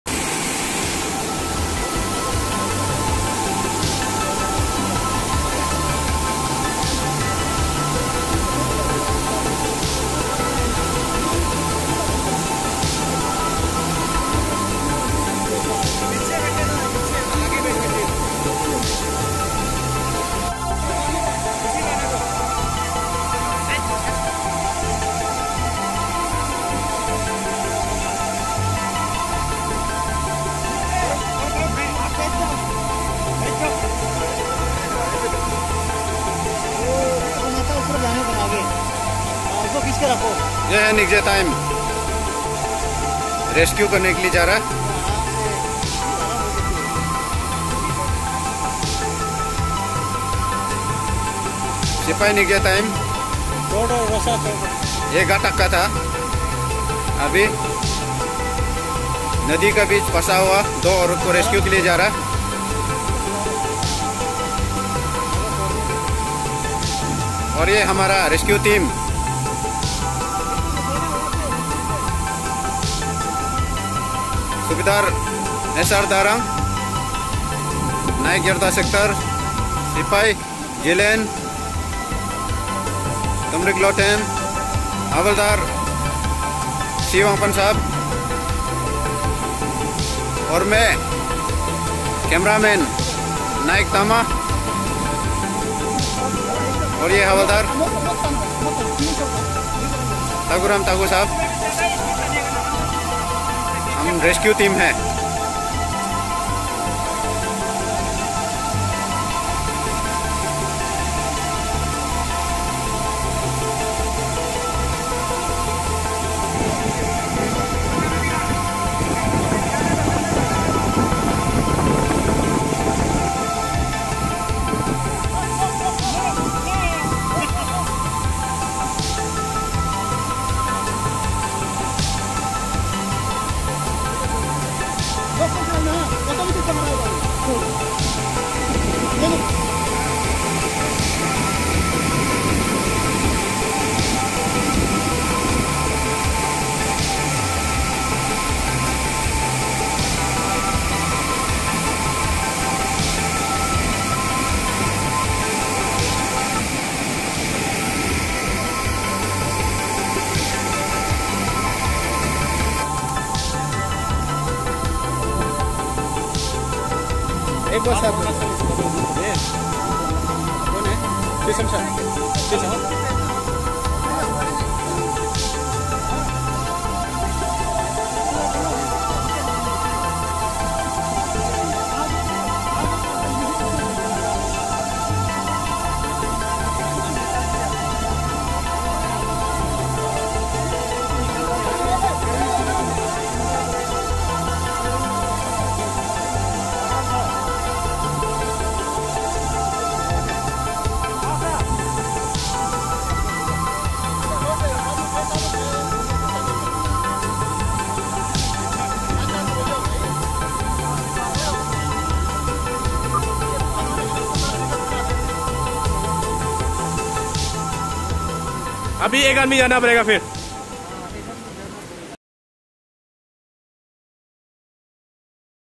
Arunachal Scout team rescues civilians trapped in flash floods at Kaying
Arunachal Scout team led by Subedar Soitoram Darang rescued the two villagers
The rescued person (women) has been identified as Yadom Takong and Yager Ruchi